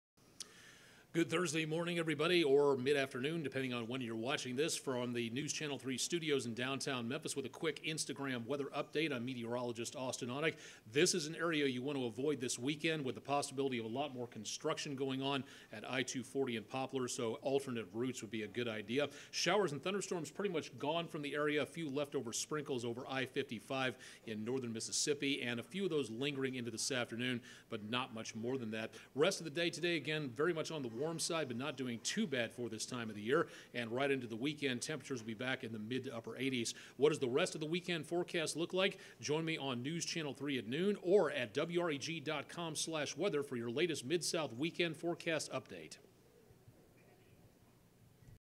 WREG-TV iNSTAWEATHER FOR FRIDAY JULY 27 2018
WREG Meteorologist Austen Onek takes a quick look at the forecast for Friday, July 27, and into the weekend.